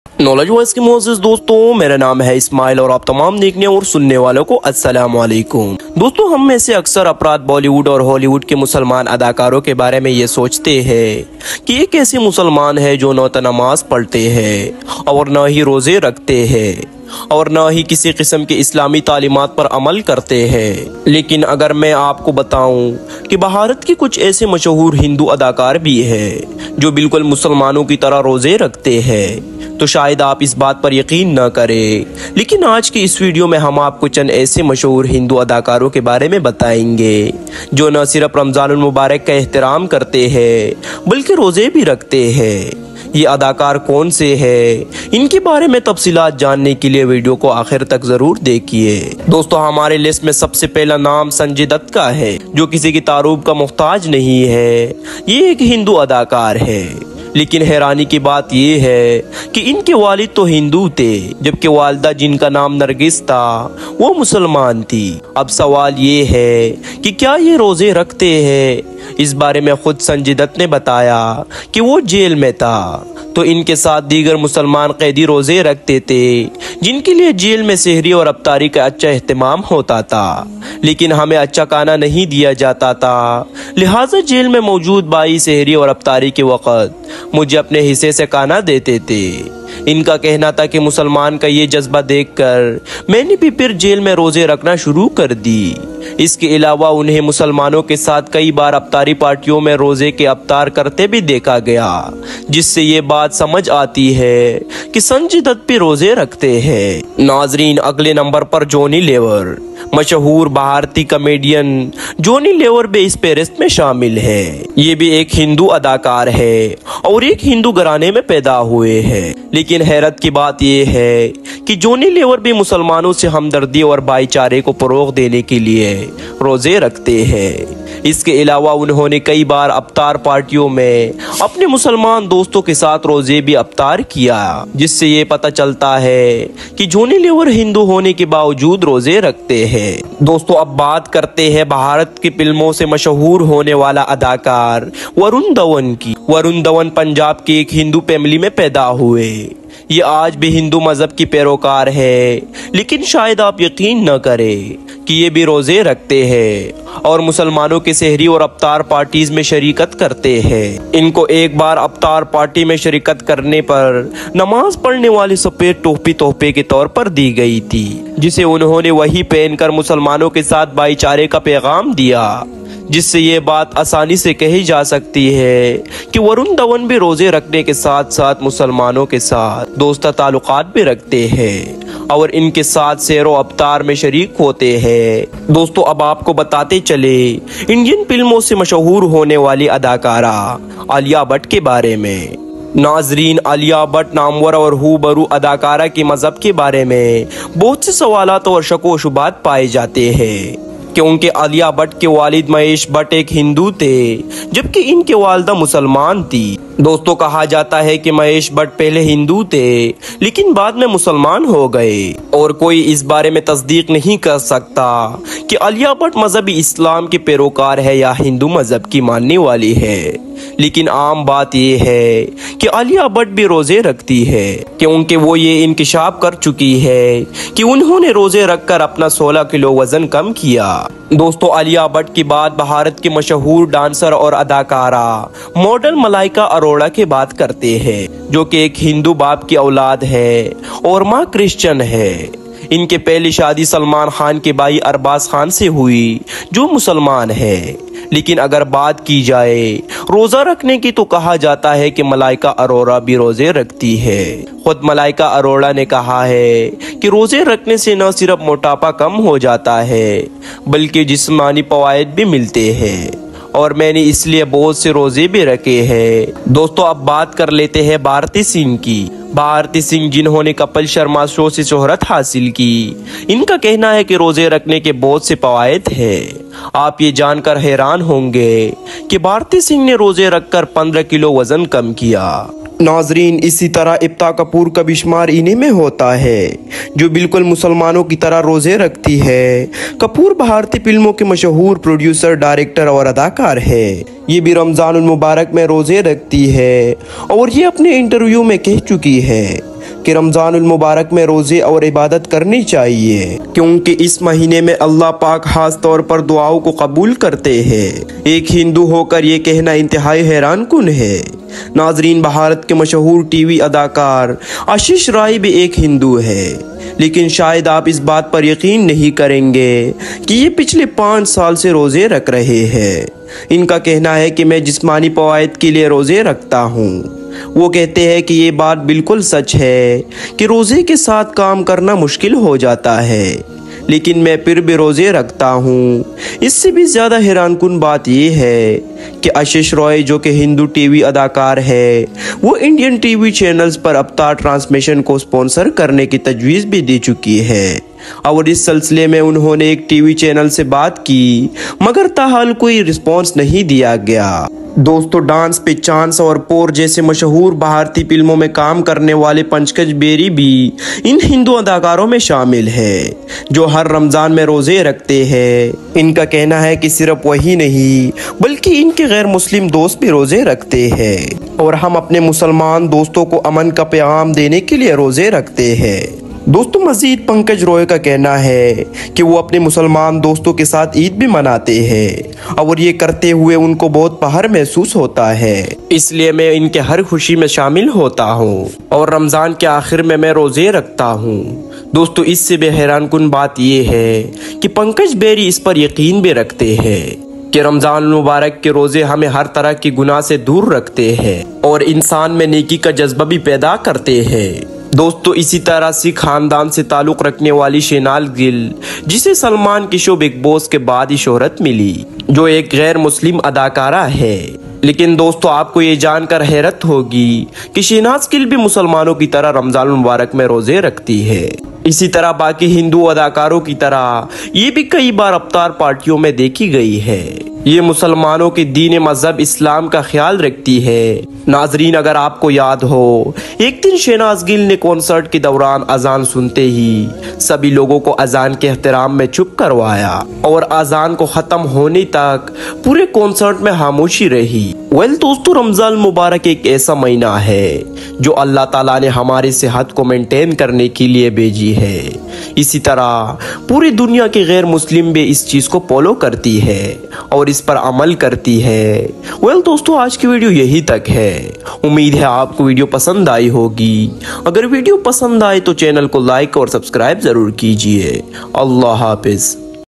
Famous Indian Actors who Fasting In Ramadan l Bollywood Hindu Actors Fasting
Topic.
In this video we talked about the Famous Bollywood Hindu Actors who are Fasting in Ramadan and follow Islamic rules in Ramadan and share the benefit of taking fast of Ramazan and share aftar to their Muslim friend and believe in fasting of Ramadan and much more interesting and informative things in this video.

Other informative video.